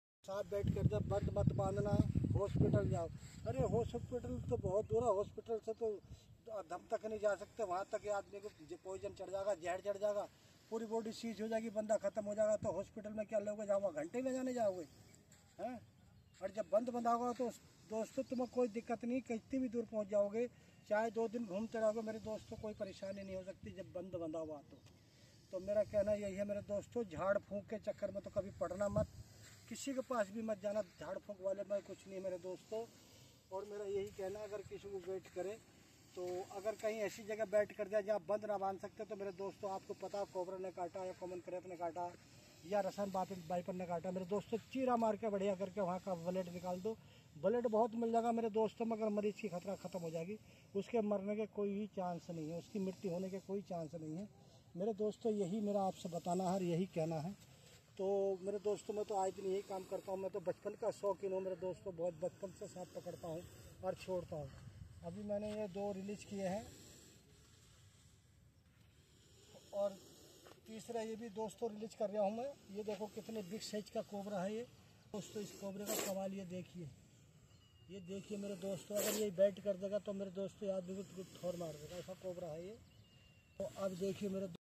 सांप काट ले तो तुरंत करें उसका उपचार | सांप काटना / सर्प दंश 🐍 😱 biggest snake rescue in india
सांप काट ले तो तुरंत करें उसका उपचार | सांप काटना / सर्प दंश 🐍 😱 biggest snake rescue in india | @ProtectionSnake